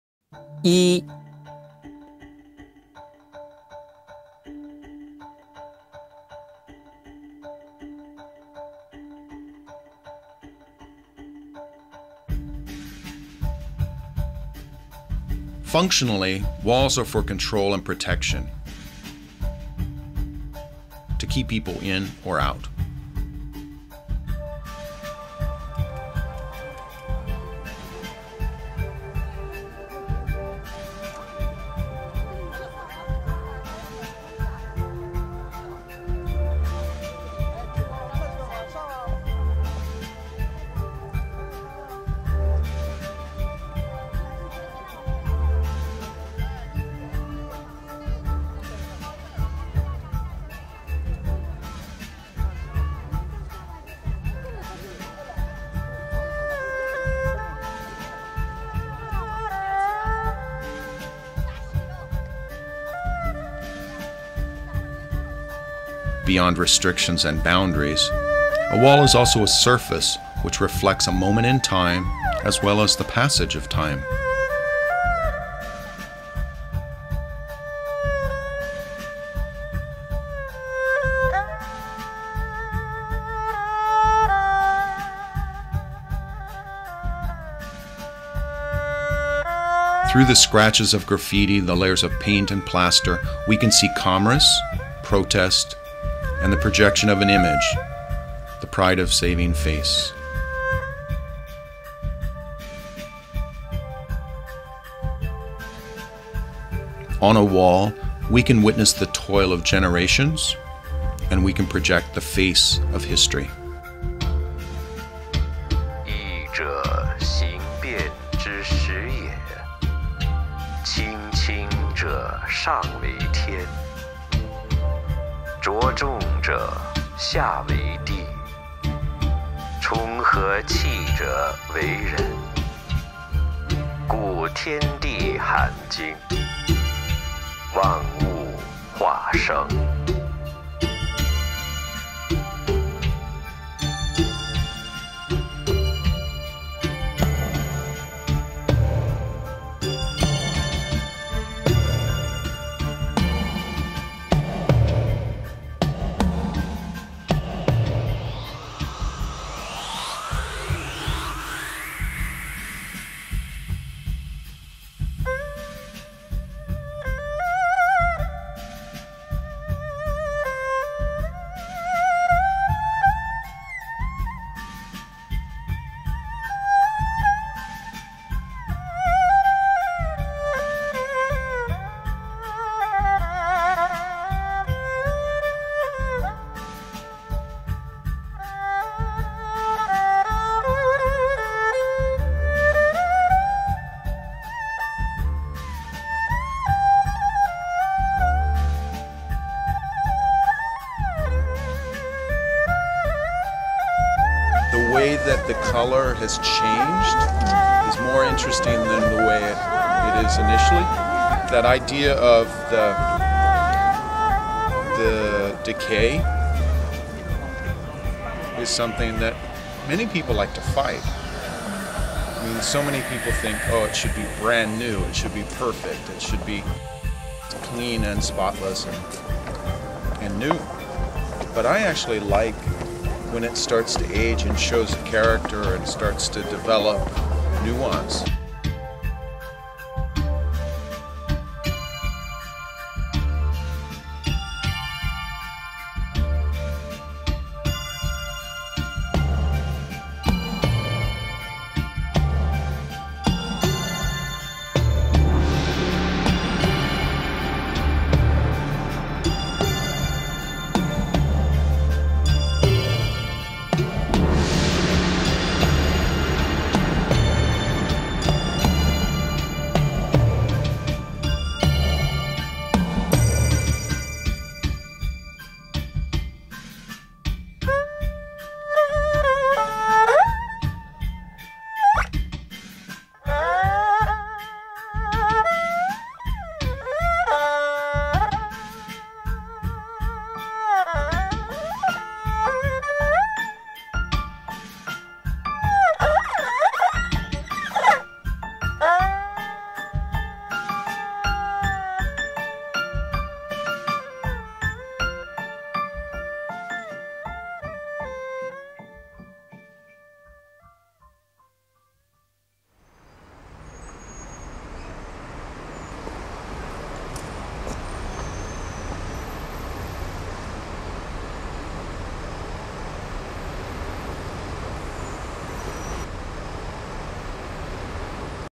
1 - China - Asia - Travel - Contemporary Art - Lifestyle Art Project - Steven P. Perkins
Part 1 of 8, WALL, Lifestyle Art Project: China. WALL considers divisions & surfaces, decay & rennovation, value of the new vs. value of the old